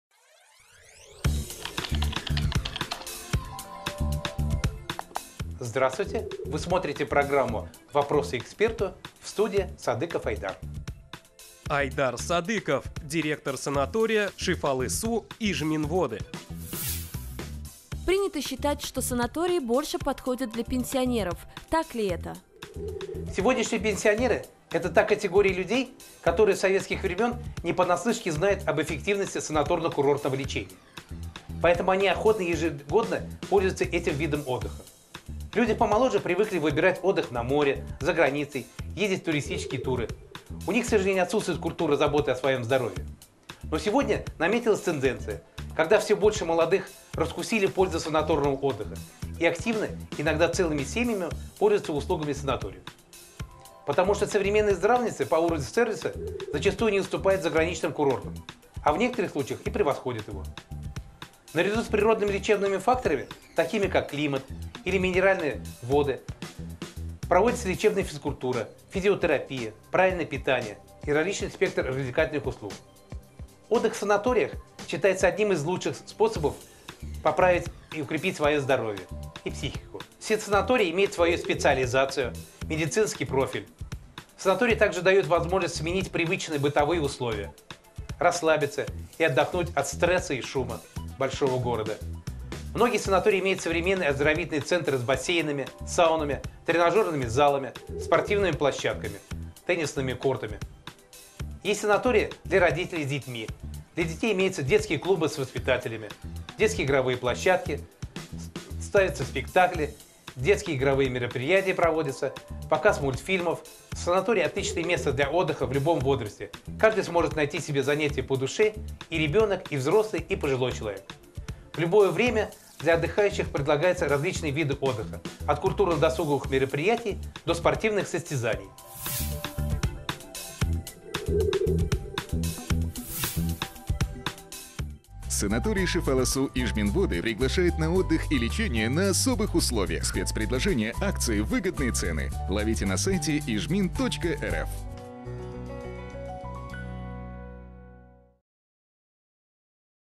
Вопрос эксперту (Ижминводы 5) - Айдар Садыков
Принято считать, что санатории больше подходят для пенсионеров, так ли это?